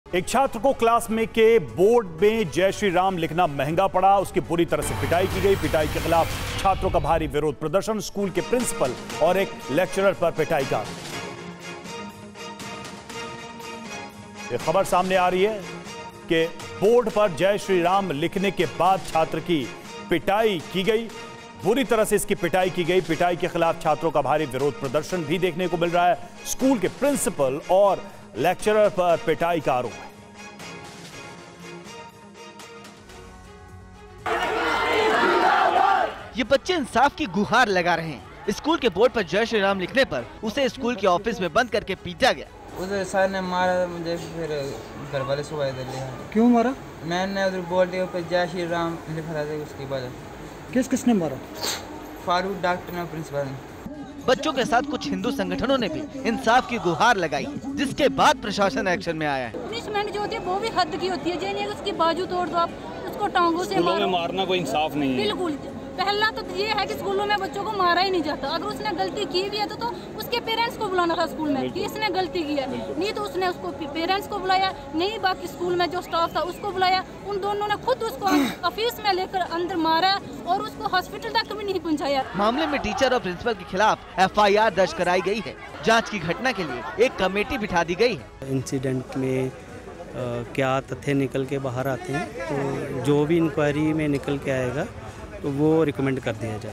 Jammu-Kashmir: छात्र ने Class Board पर लिखा 'जयश्री राम'... प्रिंसिपल और एक लेक्चरर पर पिटाई का आरोप
एक छात्र को क्लास के बोर्ड में 'जयश्री राम' लिखना महंगा पड़ा, उसकी बुरी तरह से पिटाई की गई. पिटाई के खिलाफ छात्रों का भारी विरोध प्रदर्शन. स्कूल के प्रिंसिपल और एक लेक्चरर पर पिटाई का आरोप.